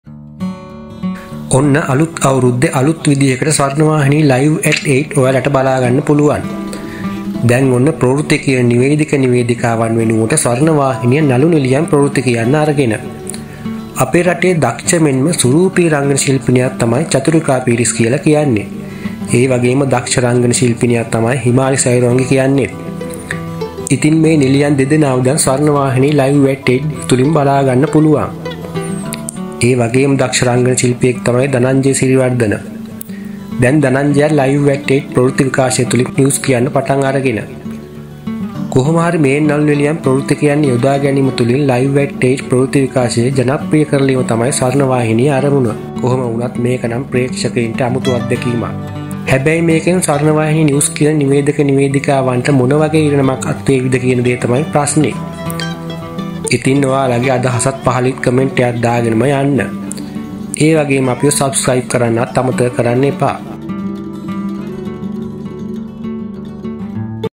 ප්‍රවෘත්ති කියන්න දඟලන අපේ නිළියන්/Our actresses came to tell the news
Our actresses came to tell the news, chathurika piris himali sayurangi,danajaya siri wardana